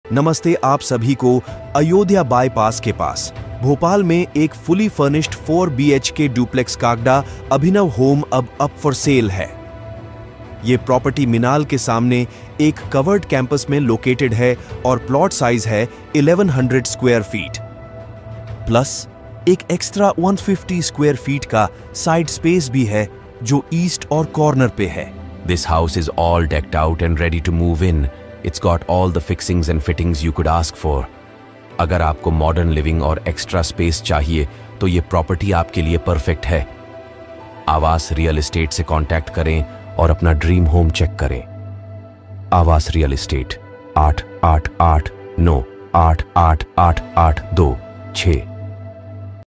4BHK Duplex For Sale Near Ayodhya Bypass Road Bhopal Madhya Pradesh
4Bhk Duplex For Sale Near Ayodhya Bypass Road Bhopal Madhya Pradesh.
For More Details Contact AAWAAS REAL ESTATE.